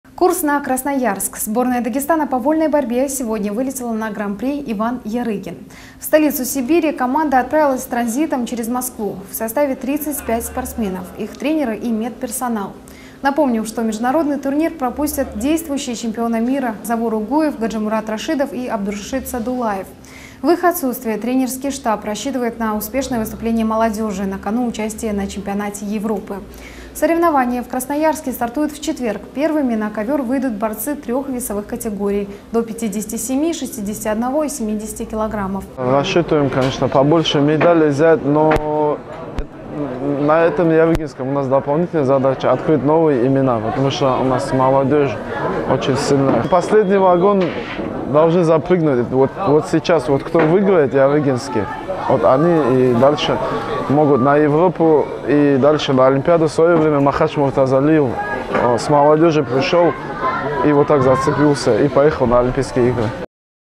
Ярыгинский 2020 Дагестанские борцы вылетели в Красноярск
Курс на Красноярск. Сборная Дагестана по вольной борьбе сегодня вылетела на Гран-при Иван Ярыгин. В столицу Сибири команда отправилась транзитом через Москву. В составе - 35 спортсменов, их тренеры и медперсонал. Напомним, что международный турнир пропустят действующие чемпионы мира Завур Угуев, Гаджимурад Рашидов и Абдулрашид Садулаев. В их отсутствие тренерский штаб рассчитывает на успешное выступление молодежи. На кону - участие на чемпионате Европы. Соревнования в Красноярске стартуют в четверг, первыми на ковёр выйдут борцы трёх весовых категорий - до 57, 61 и 70 кг.